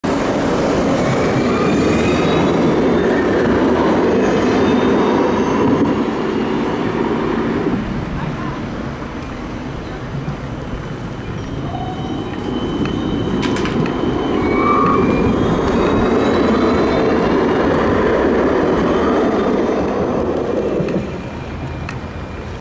GVB Siemens Combino 2141 & 2138 tramhalte Kattenburgerstraat te Amsterdam | tramlijn 26
De video is gemaakt op 22 april 2016 bij de tramhalte Kattenburgerstraat (Piet Heinkade) te Amsterdam, waar GVB tramlijn 26 rijdt.